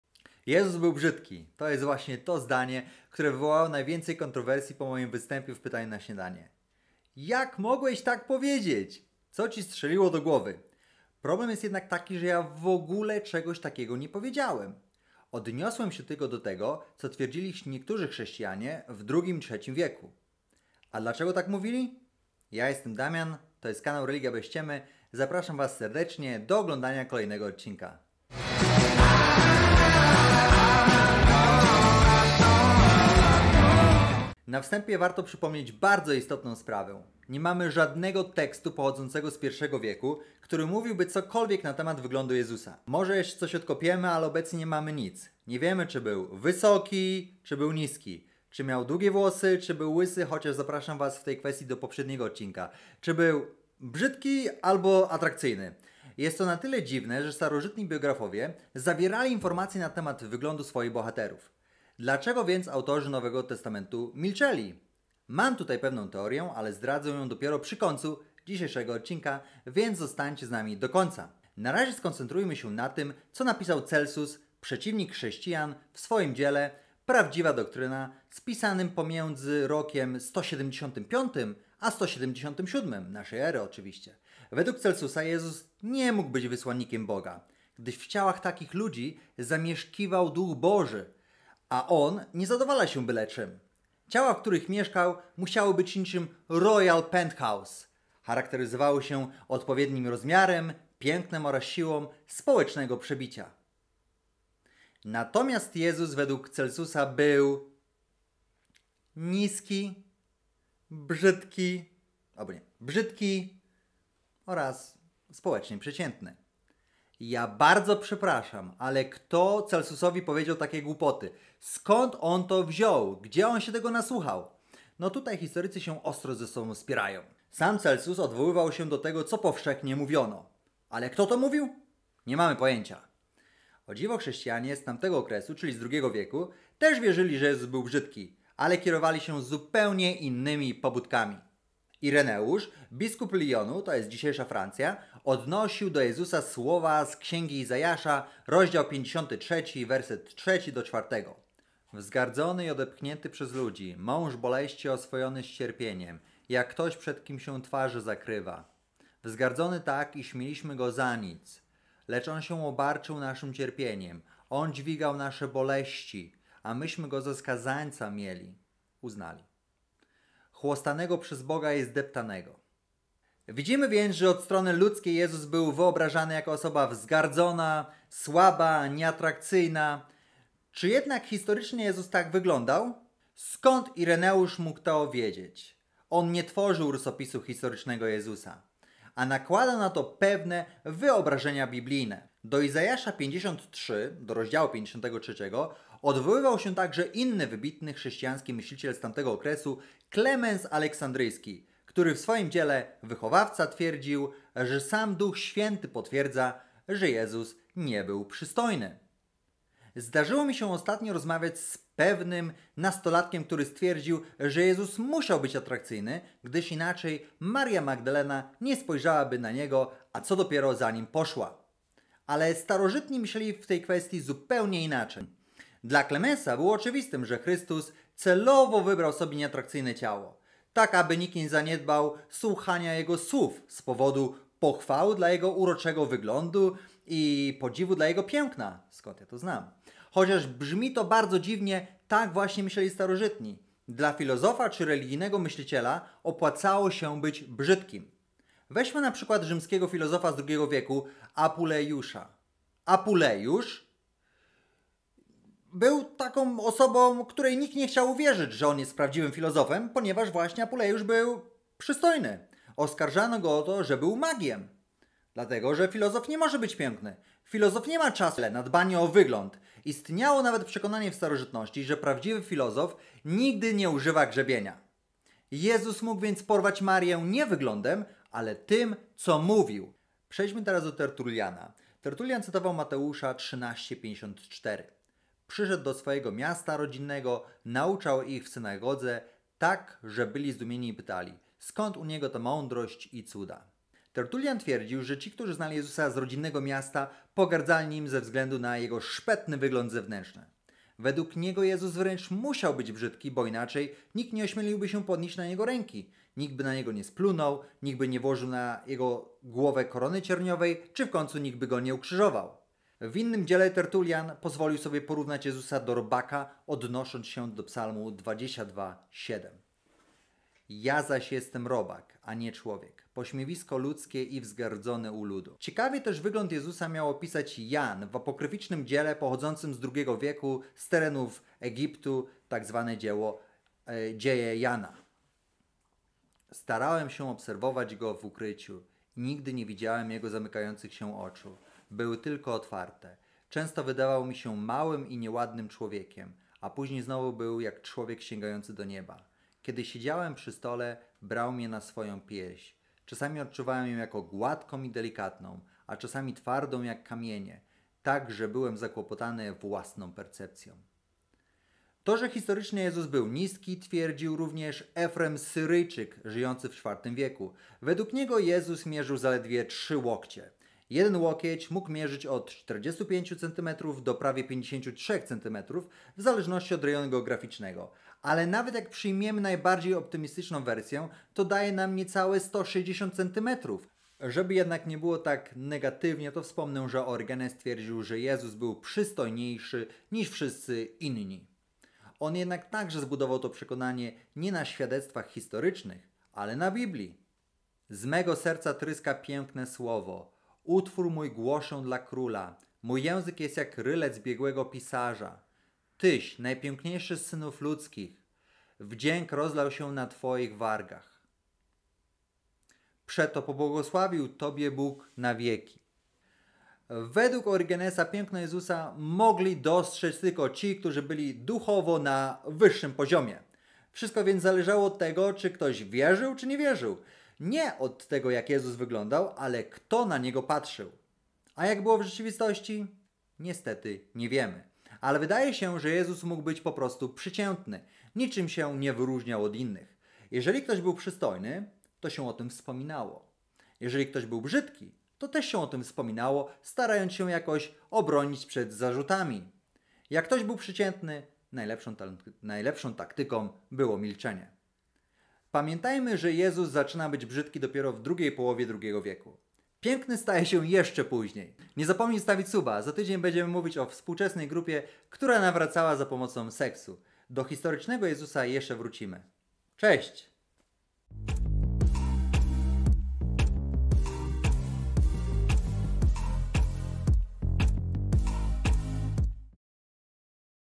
🤯 CZY JEZUS BYŁ BRZYDKI? / WAS JESUS UGLY?
Czas na drugi z kolei film z serii aparycyjnej. Dziś zastanawiamy się nad pytaniem: „Czy Jezus był brzydki?”🤔

❗️UWAGA - poniżej znajdują się istotne edycje do filmiku, których już nie mogliśmy zamieścić w samym wideo:

1) Uprzedzając Wasze skargi, zażalenia, albo ewentualne pytania - chcemy wytłumaczyć (bo w słowotocznym przypływie myśli u Damiana, mogło to nie bardzo wybrzmieć), że absolutnie nie sugerujemy, że wzrost ma cokolwiek wspólnego z wyglądem! Jednak - pamiętajcie, że w tym filmiku analizujemy dowody historyczne, czyli między innymi to, że nieprzypadkowo Celsus w jednym zdaniu wymienił cechy: brzydki, społecznie niewyróżniający się i... niski. Wówczas miało to zupełnie inne znaczenie, niż dziś 🙂

2) Wymieniając kolejny słowotoczny błąd Damiana... *minuta ciszy*: cytowanie z księgi Izajasza powinno sięgać zdania wyżej (bo to właśnie w nim jest cokolwiek o wyglądzie Jezusa), które brzmi:
"Nie miał On wdzięku, ani też blasku, aby na Niego popatrzeć, ani wyglądu, by się nam podobał."

☕ Chcesz nas wesprzeć? Postaw nam kawusię i napisz coś miłego!

Nie zapomnijcie zostawić suba! 🤍